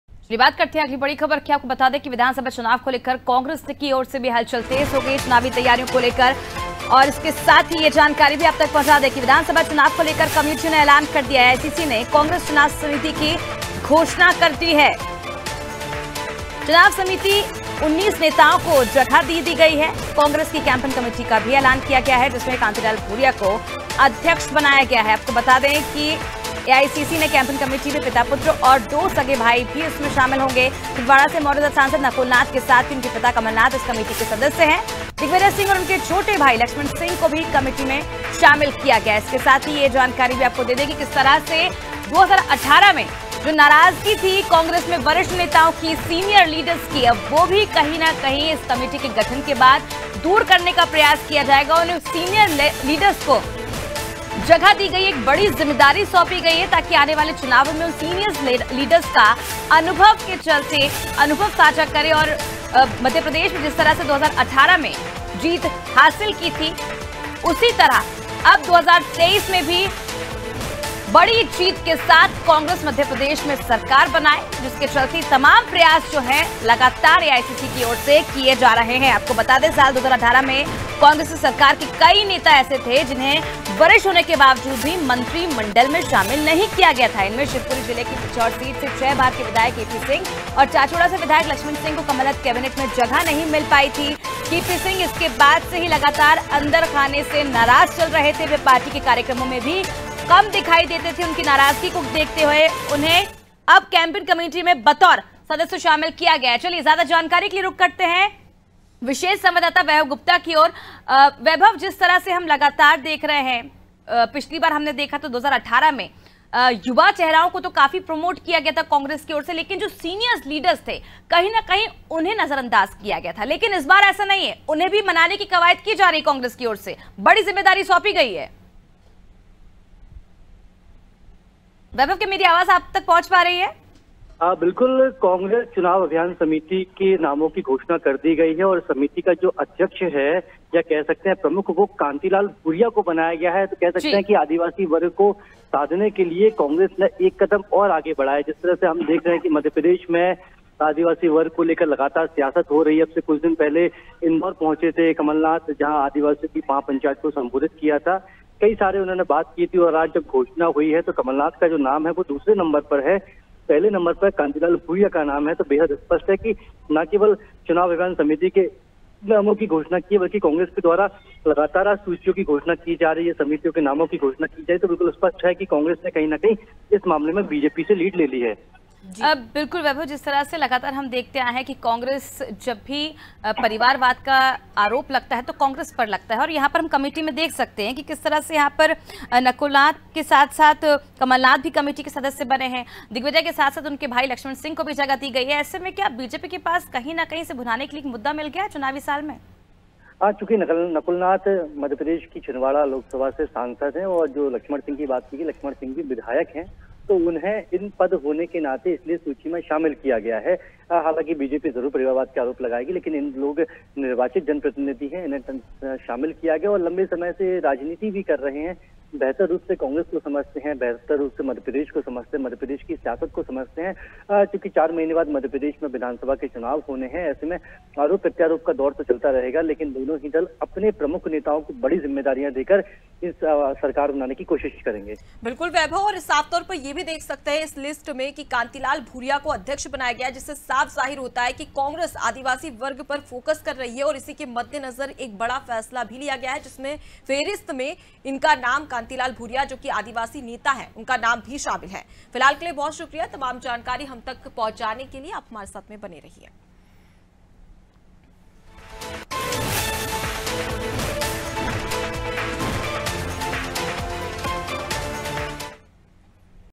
Bhopal : एमपी कांग्रेस में कमेटियों का ऐलान जारी | BREAKING NEWS || Anaadi Tv
भोपाल:एमपी कांग्रेस में कमेटियों का ऐलान जारी
विधानसभा चुनाव को लेकर कमेटियों का ऐलान
AICC ने की कांग्रेस चुनाव समिति की घोषणा
कमलनाथ बने चुनाव समिति के अध्यक्ष
चुनाव समिति 19 नेताओं को मिली जगह
कांग्रेस की कैंपेन कमेटी का भी एलान
कांतिलाल भूरिया बनाए गए अध्यक्ष
कांग्रेस ने आदिवासियों पर किया फोकस
कमेटी में 34 नेताओं को मिली जगह
कमलनाथ,नकुलनाथ,दिग्विजय भी शामिल

COMING SOON
 DEN, AIRTEL